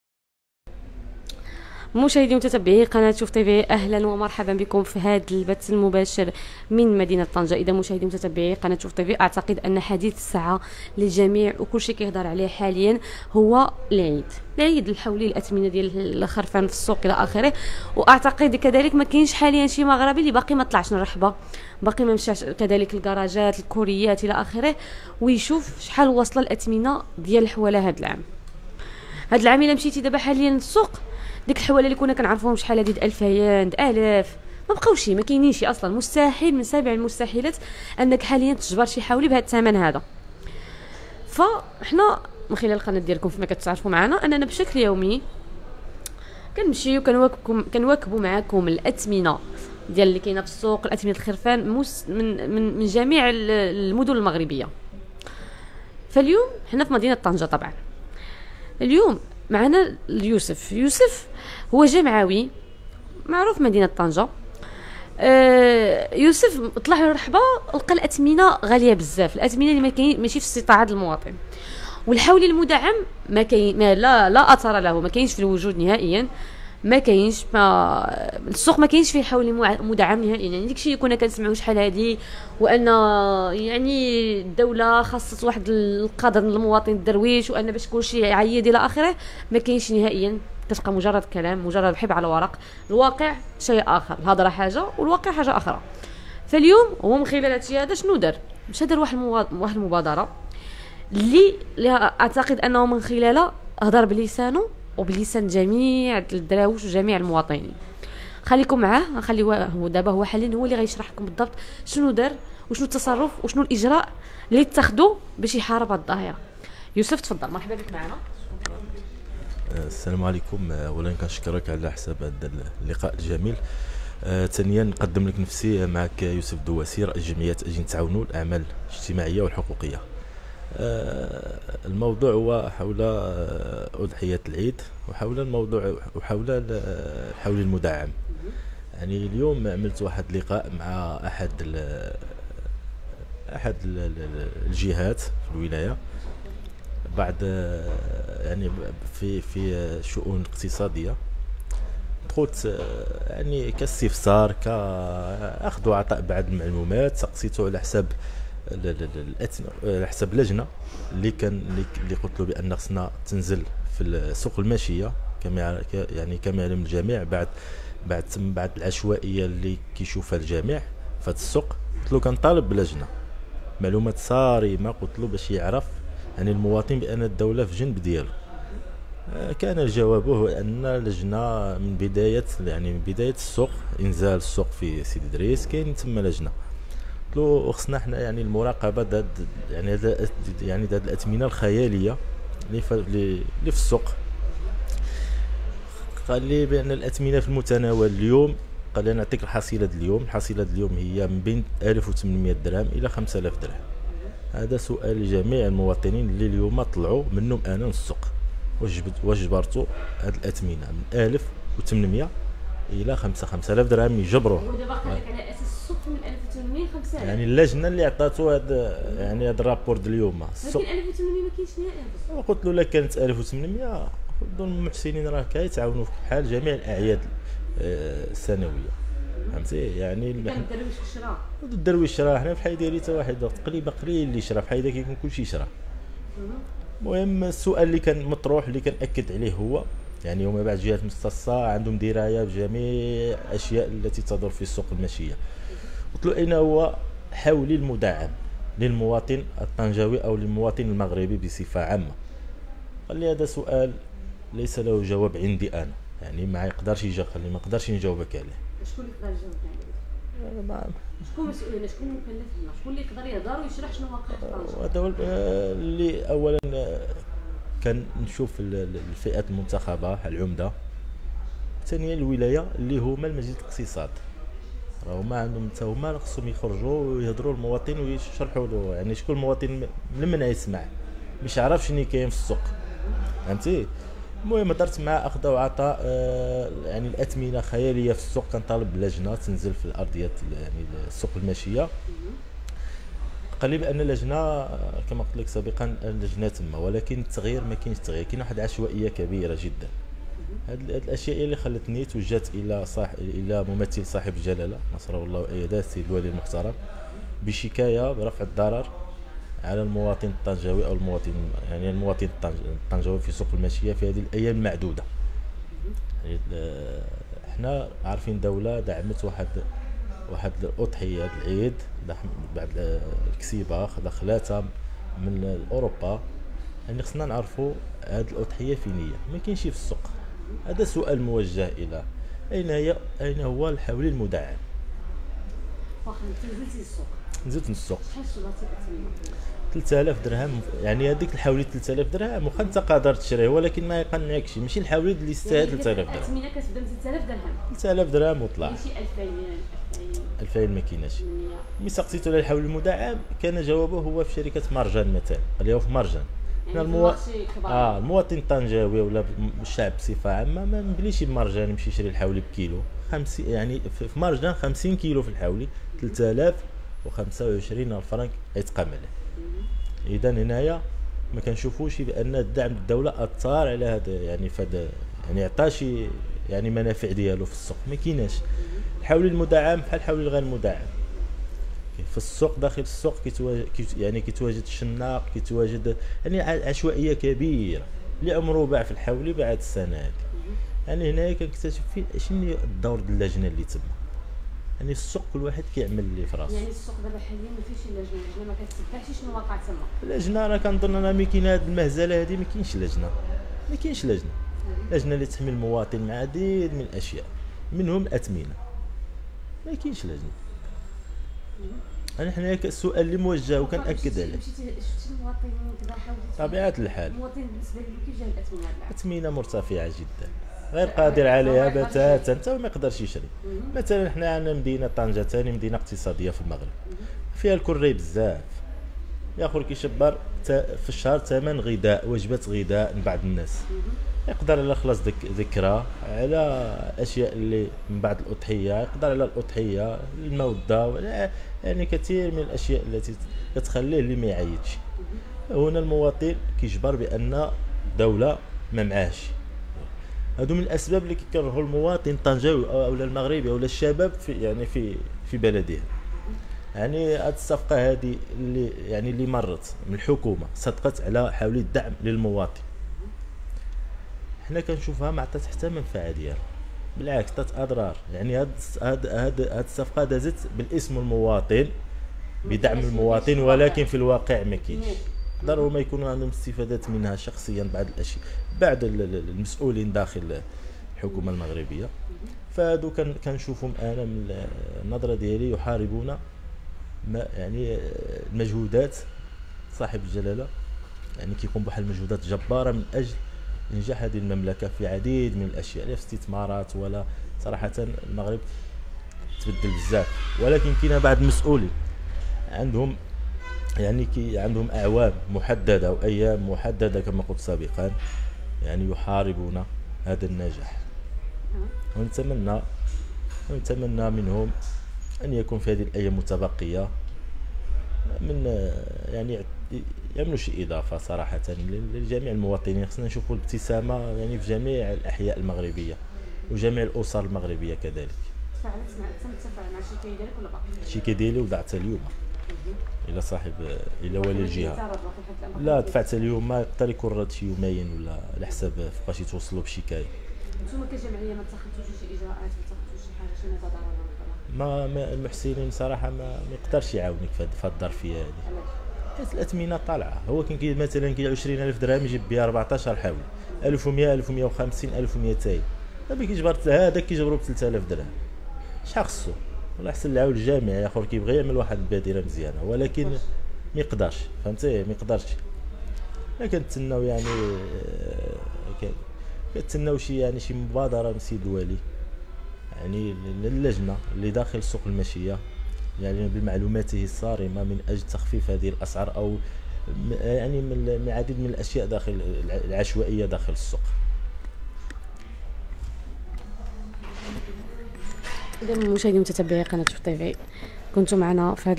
فين ديرتو الحولي المدعم؟ "يوسف" دخل طول وعرض فالمسؤولين لي حرمو المواطن الدرويش من الحولي هاد العيد
مباشرة من طنجة: فين ديرتو الحولي المدعم؟ "يوسف" دخل طول وعرض فالمسؤولين لي حرمو المواطن الدرويش من الحولي هاد العيد.. لايف قنبلة